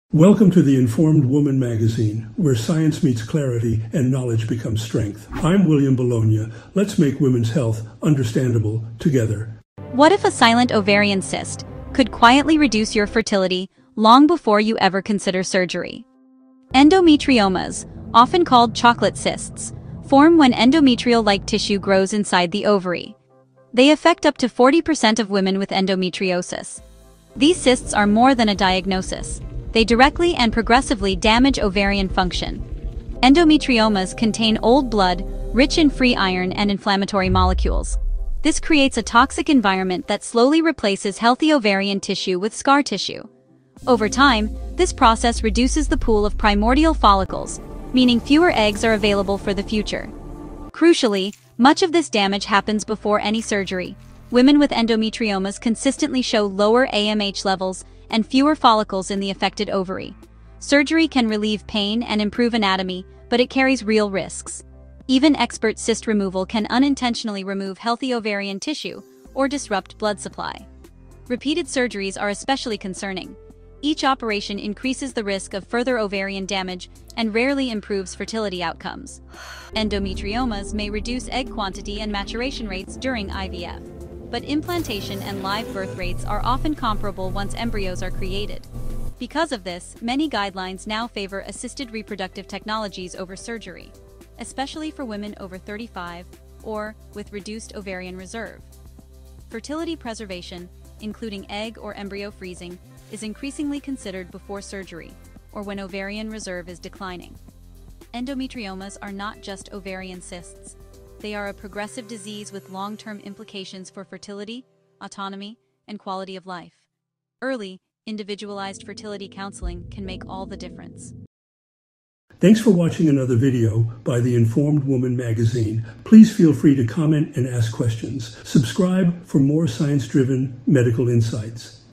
Preserving Fertility Despite Ovarian Endometriomas
Endometriomas, often called “chocolate cysts”, can directly damage the ovary long before any surgery is performed. These cysts create a toxic, inflammatory environment that replaces healthy ovarian tissue with fibrosis, leading to lower AMH levels and reduced egg supply. While surgery can relieve pain, it also carries a real risk of further ovarian reserve loss, especially with repeat procedures. Because of this, many guidelines now favor IVF and early fertility counseling over repeated surgery. Egg or embryo freezing may help preserve reproductive options in women with endometriomas, particularly when ovarian reserve is declining.